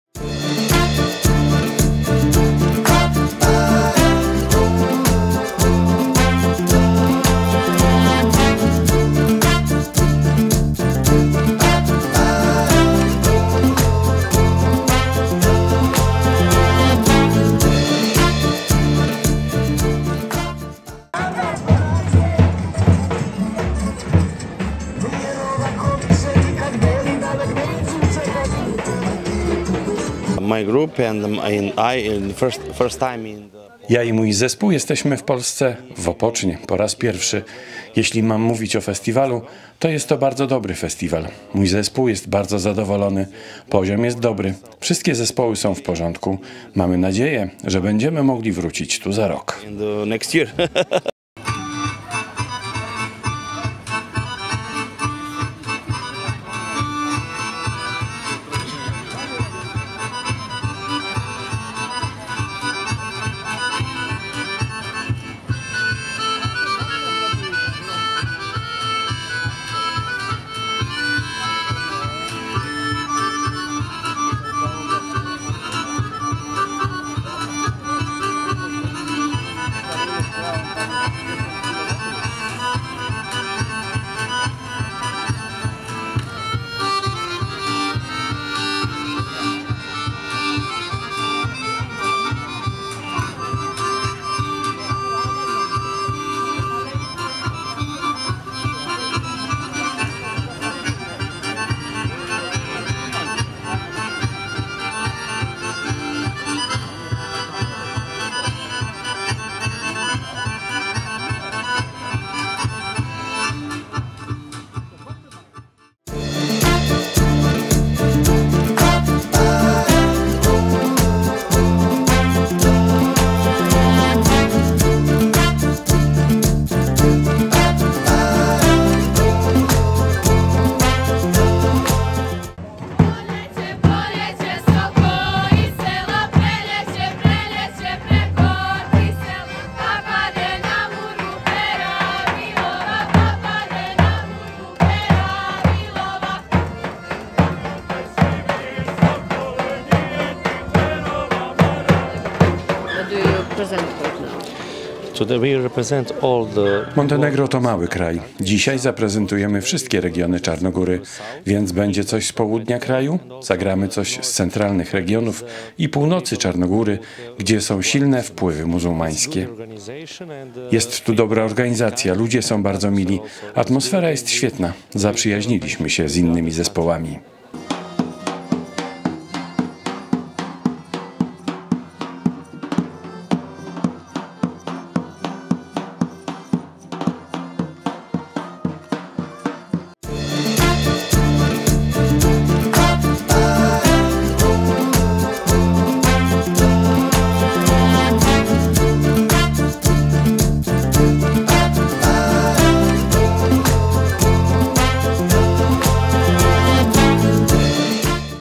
IX Międzynarodowy Festiwal Folklorystyczny "Opoczno 2023" cz.6
Występ zespołów z Czarnogóry i Serbii.